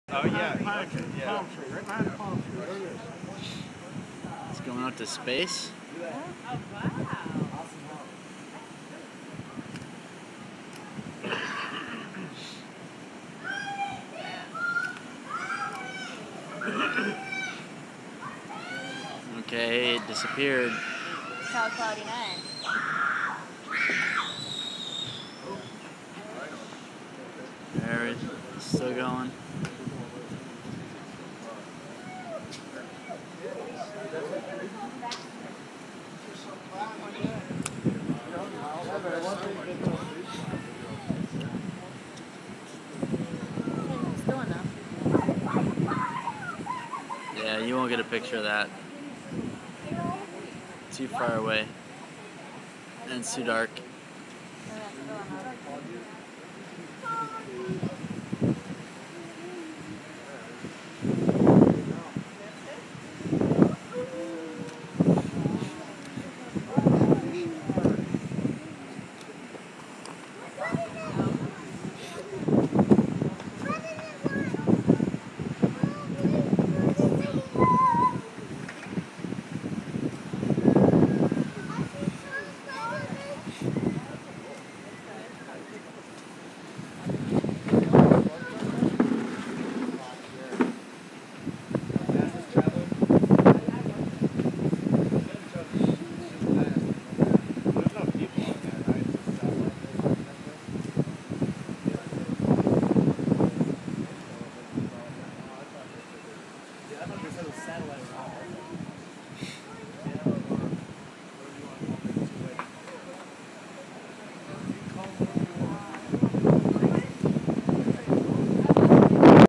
NASA Launches Rocket Into Space
While I was at Disney World in Orlando, FL, I had the chance to see NASA launch a rocket into space.